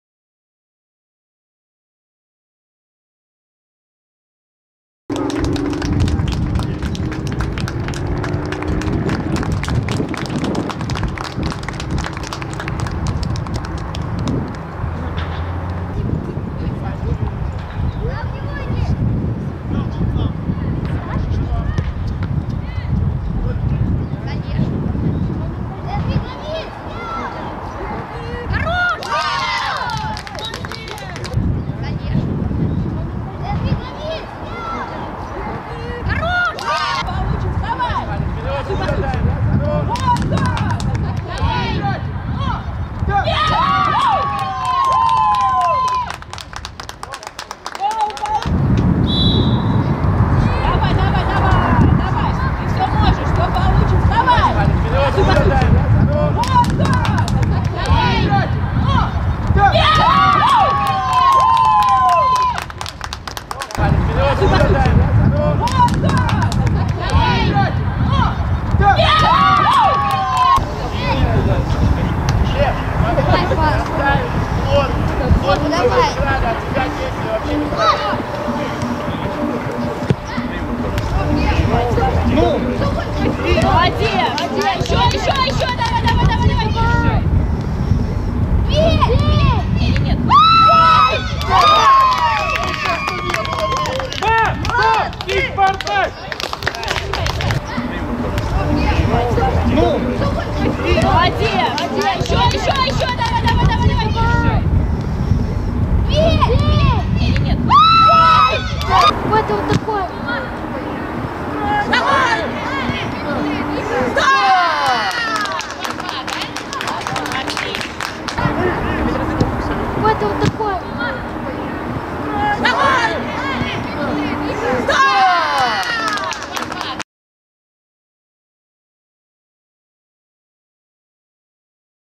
«Спартак» — «Локомотив» (команды 2014 г. р.) — 2:3
Товарищеский матч
Команды 2014 года рождения
«Спартак» — «Локомотив» — 2:3 (1:1)
Голы: Шеболдасов, Жигалов.